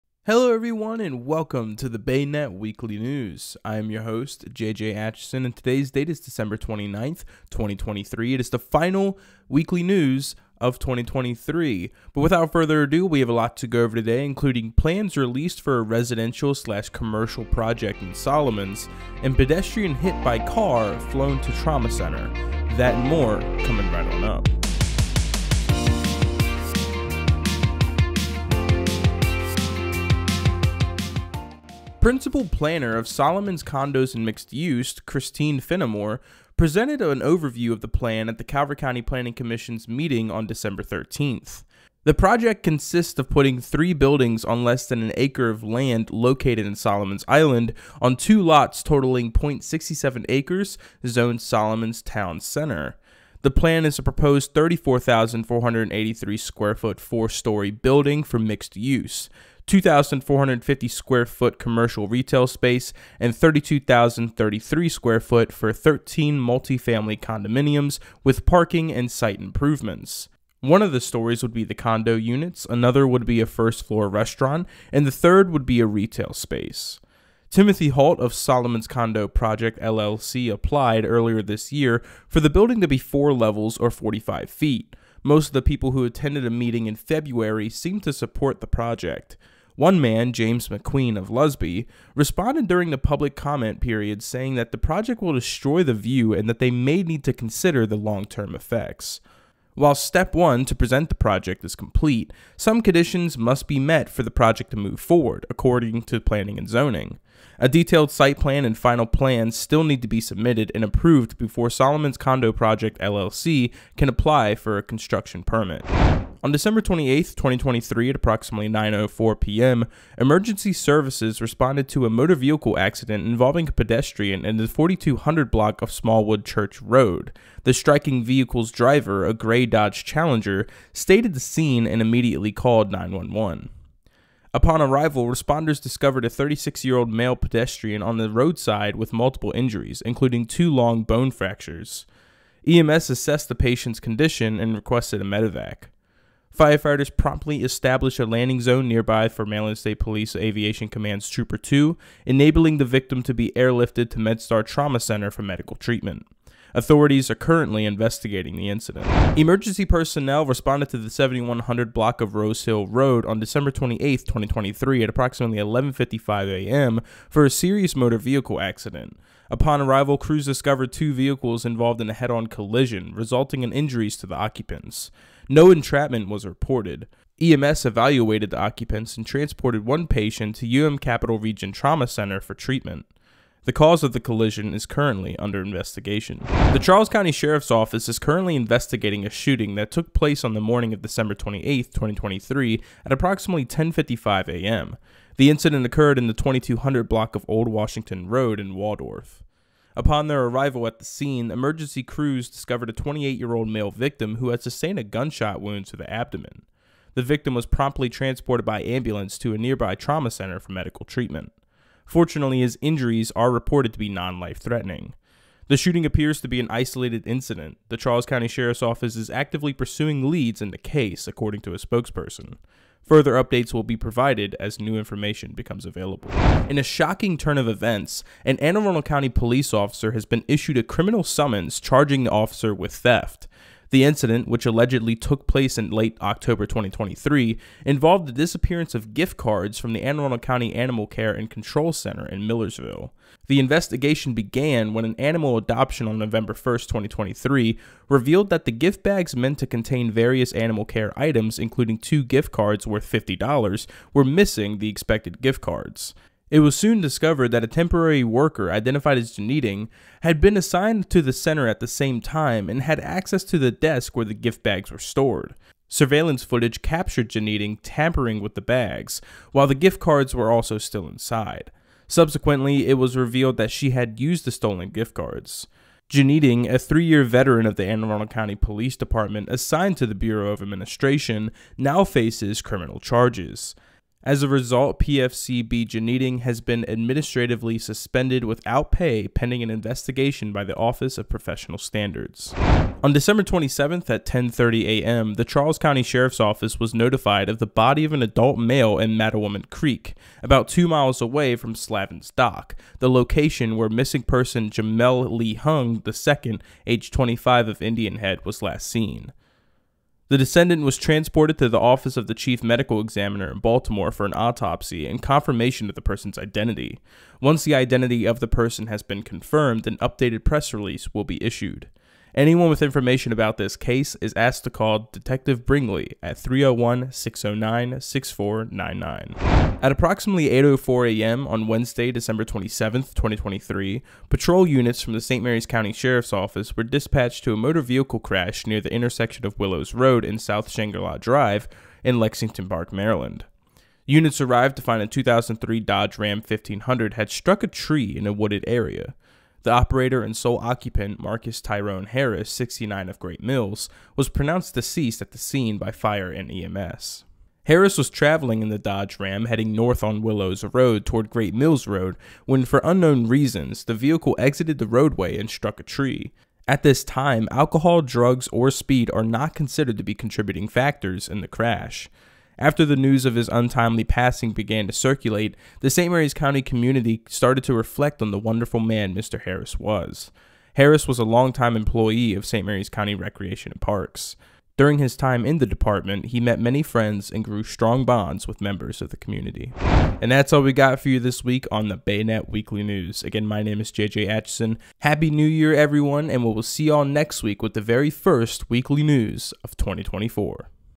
The BayNet Weekly News - December 29, 2023
Plans Released For A Residential/Commercial Project In Solomons - @eleased-for-a-residential-commercial-project-in-solomons/

Pedestrian Hit By Car, Flown To Trauma Center - @ian-hit-by-car-flown-to-trauma-center/

One Injured After Head-On Collision In Port Tobacco - @ured-after-head-on-collision-in-port-tobacco/

Waldorf Man Shot In Abdomen, Police Investigating - @-man-shot-in-abdomen-police-investigating/

Officer Suspended Without Pay After Charged With Theft At Anne Arundel Animal Care And Control Center - @-suspended-without-pay-after-charged-with-theft-at-anne-arundel-animal-care-and-control-center/

Body Of Adult Male Located In Mattawoman Creek; Possibly Missing 25-Year-Old Man - @-adult-male-located-in-mattawoman-creek/

69-Year-Old Great Mills Man Killed In Fatal Crash - @-old-great-mills-man-killed-in-fatal-crash/